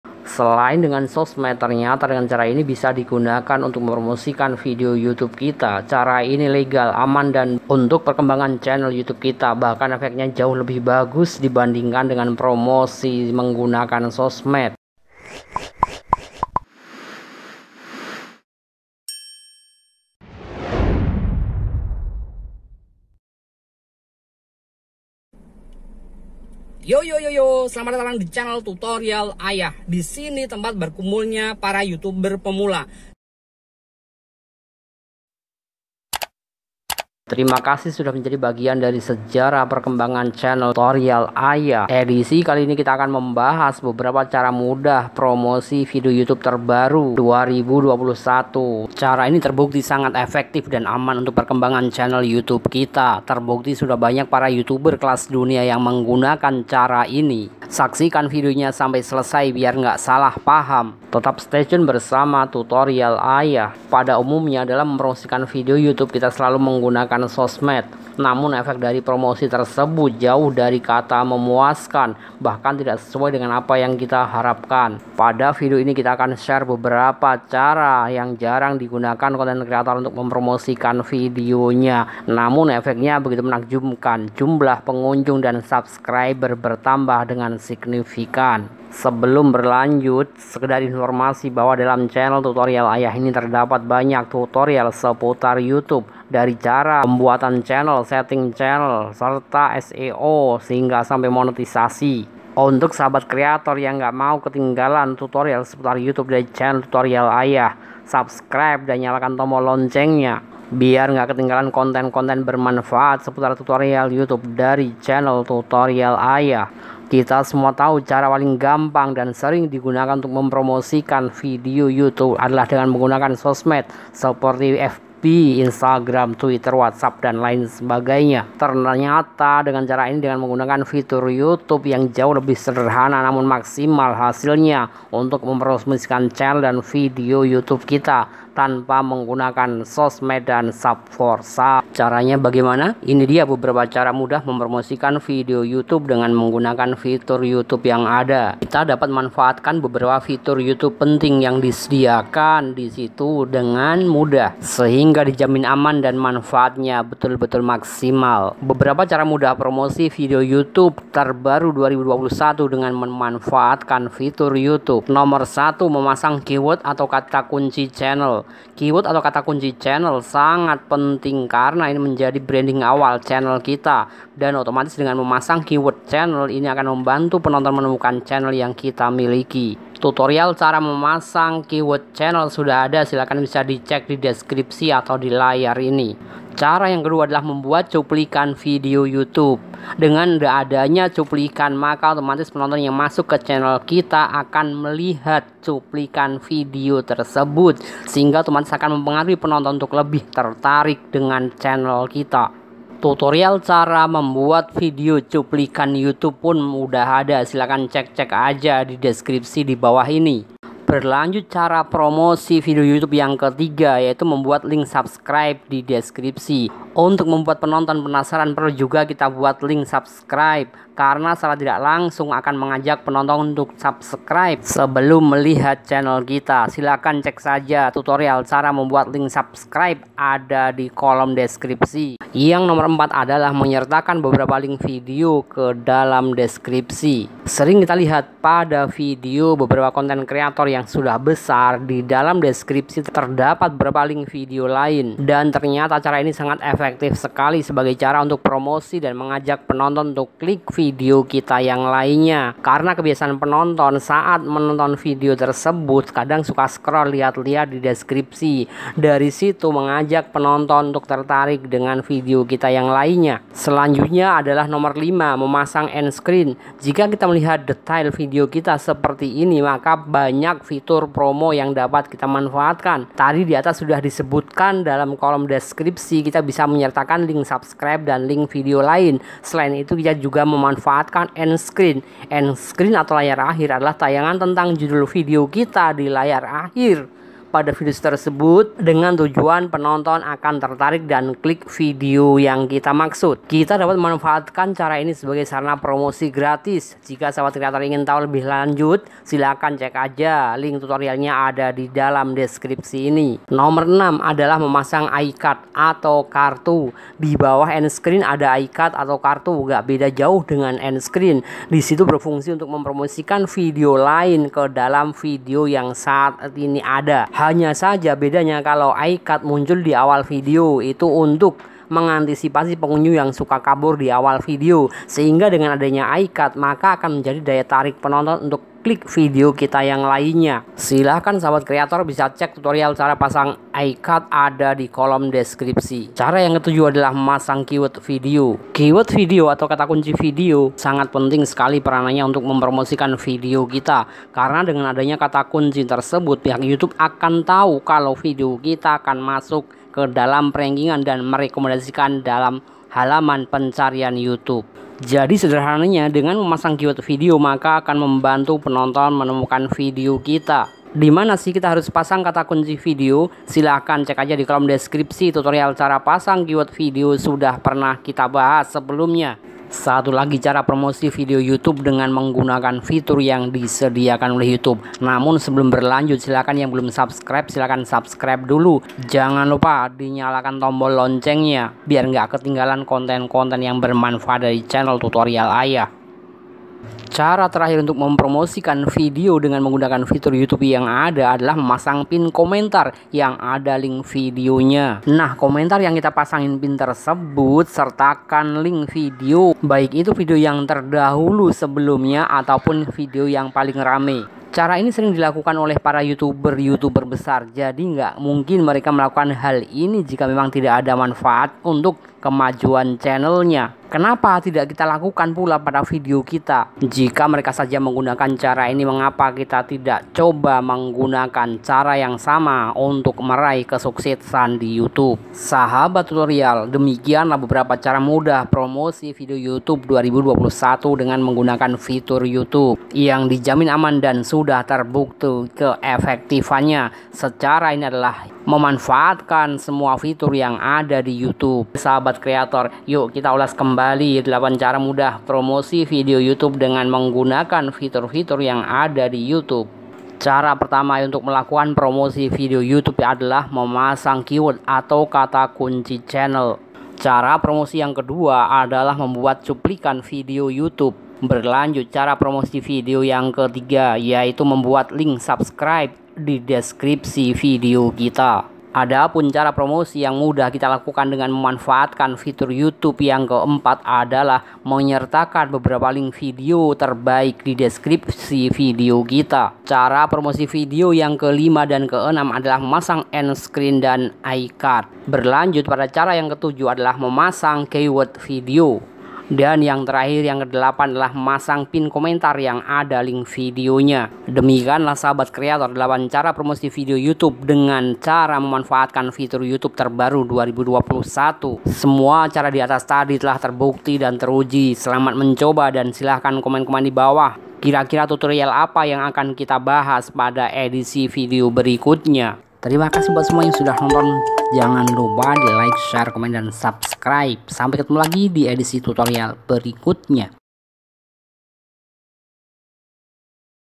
CARA MUDAH PROMOSI VIDEO YOUTUBE GRATIS || TERBUKTI AMPUH
Halo Sobat Tutorial
Kali ini Tutorial Ayah akan membahas tentang cara promosi video youtube, gratis gak pake biaya apapun.
cara promosi video yang kita bahas dalam video ini semua sesuai dengan ketentuan youtube sendiri jadi dipastikan aman dan bisa meningkatkan pendapatan youtube kalian.
simak terus videonya sampai selesai ya gengs. biar gak salah paham

Cara Promosi Video Youtube || Gratis
Mudah Dan Terbukti menghasilkan

Ini beberapa link video tutorial   seputar youtube

Thanks

Tutorial Ayah